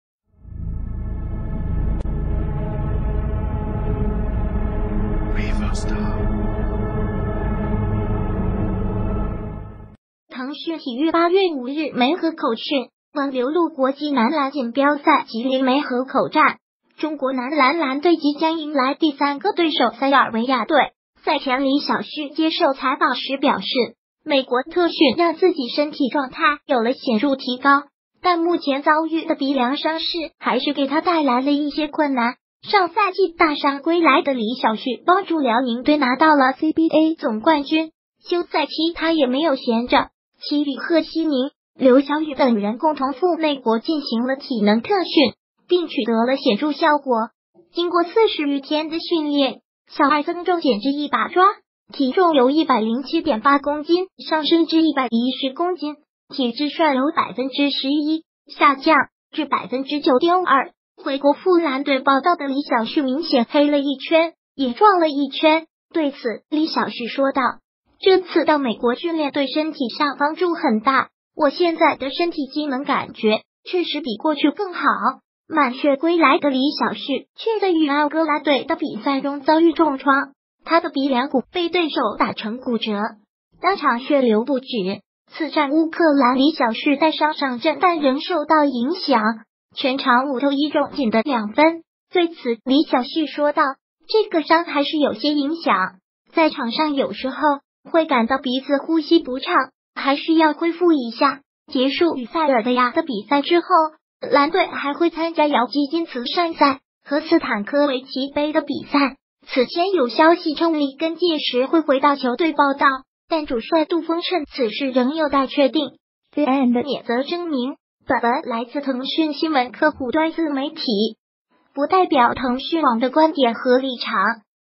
李晓旭：美国特训帮助大 鼻梁骨折影响呼吸
)腾讯体育8月5日梅河口讯（文/刘陆）国际男篮锦标赛吉林梅河口站，中国男篮蓝队即将迎来第三个对手塞尔维亚队。赛前李晓旭接受采访时表示，美国特训让自己身体状态有了显著提高，但目前遭遇的鼻梁伤势还是给他带来了一些困难。上赛季大伤归来的李晓旭帮助辽宁队拿到了CBA总冠军，休赛期他也没有闲着，其与贺希宁、刘晓宇等人共同赴美国进行了体能特训，并取得了显著效果：经过40余天的训练，“小二”增重减脂一把抓，体重由107.8公斤上升至110公斤，体脂率由11%下降至9.2%。回国赴蓝队报到的李晓旭明显黑了一圈，也壮了一圈，对此李晓旭说到：“这次到美国训练对身体上帮助很大，我现在的身体机能感觉确实比过去更好。”满血归来的李晓旭却在与安哥拉队的比赛中遭遇重创，他的鼻梁骨被对手打成骨折，当场血流不止。次战乌克兰李晓旭带伤上阵但仍受到影响，全场5投1中仅得2分。对此李晓旭说到：“这个伤还是有些影响，在场上有时候会感到鼻子呼吸不畅，还需要恢复一下。”结束与塞尔维亚的比赛之后，蓝队还会参加姚基金慈善赛和斯坦科维奇杯的比赛，此前有消息称李根届时会回到球队报到，但主帅杜锋称此事仍有待确定。theend免责声明：本文来